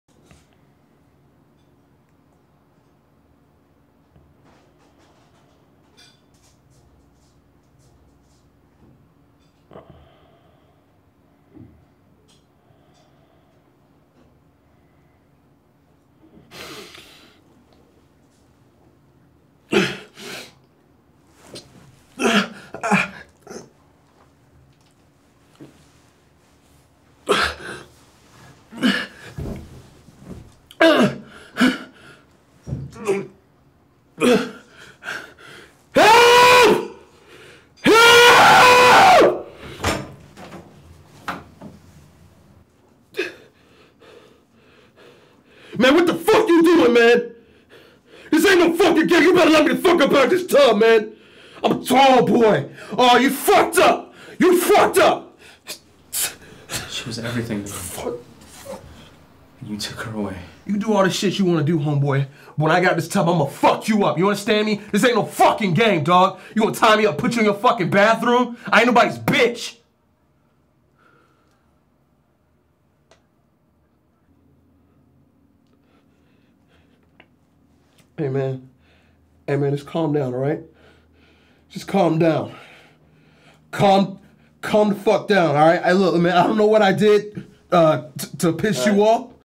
Acting / Directing Workshop | Tim Banks & YJ Gold - Bathroom Scene
Pieces of Tim Banks' acting for his acting reel. A short bathroom piece.

Directed & Edited by Nick Cabanas